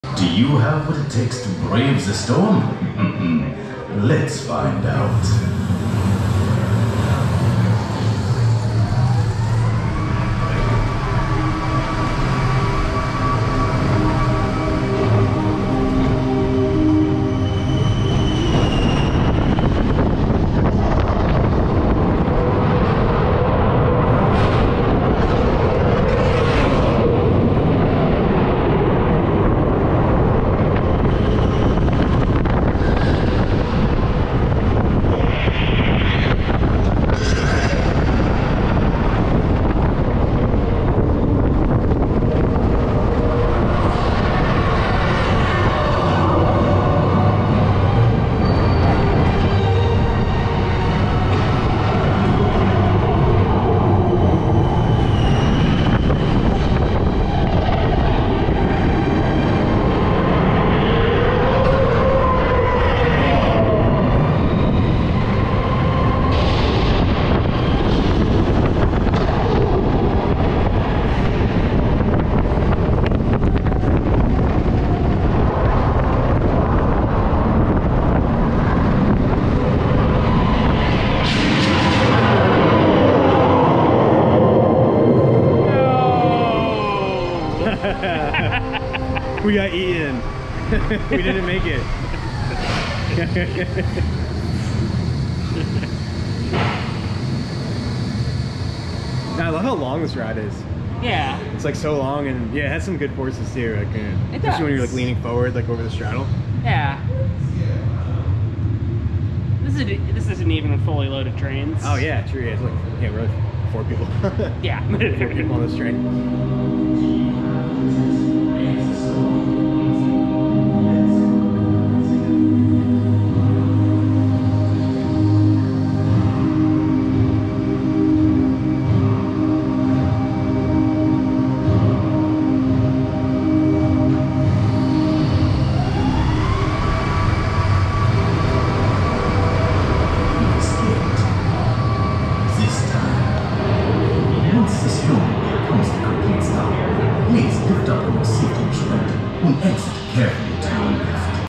DarKoaster Backrow POV - Busch Gardens Williamsburg - (Non Copyright)
Back Row POV of the all new DarKoaster at Busch Gardens Williamsburg. Feel free to use this in your own videos as long as you provide credit to this channel.

Warning: I am certainly not a great POV filmer and this is an attempt at filming a roller coaster in the pitch dark.

Channel artwork designed by Casie Jo Johnson! @blockbrakes